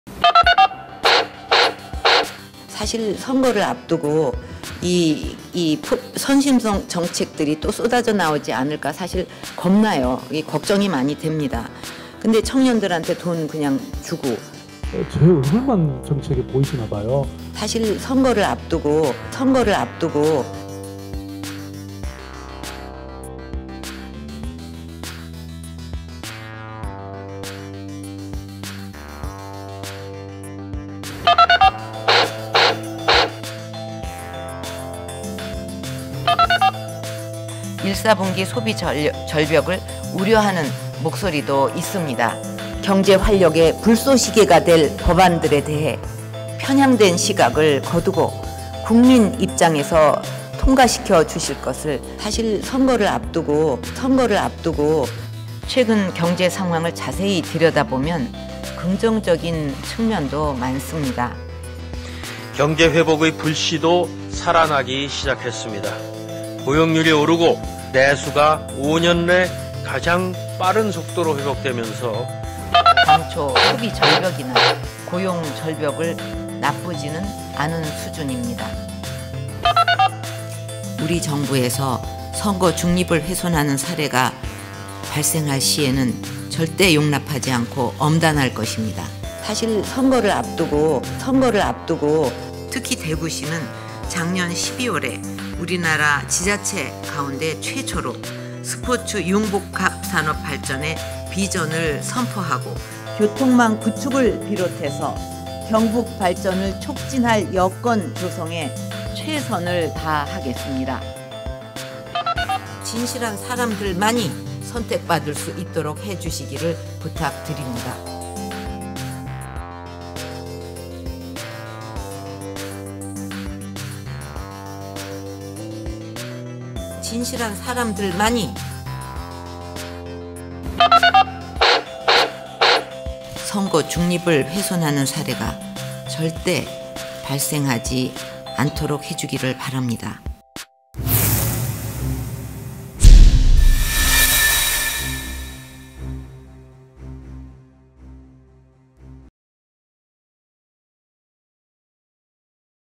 박근혜 대통령의 ‘총선 시그널?’
‘비박계’가 대거 공천에 탈락한 가운데 지난 10일 대구에 이어 16일 부산을 방문한 박근혜 대통령의 파격 행보를 두고 ‘선거 개입’이라는 비판이 제기되는 등 논란이 일고 있다.

또 박 대통령은 그동안 “소비 절벽, 고용절벽”이라며 경제가 어렵다고 주장하며 국회에 관련법안 통과를 압박하고, 서울시와 성남시청이 추진한 청년 수당은 포퓰리즘이라고 맹비난해왔다.

그런데 지난 7일 청와대 수석비서관회에서 박 대통령은 “최근 경제 상황을 자세히 들여다 보면 긍정적인 측면도 많다”며 갑자기 말을 바꿨는데, 총선을 앞두고 전형적인 '내로남불'(내가 하면 로맨스 남이 하면 불륜) 행보를 보이고 있다.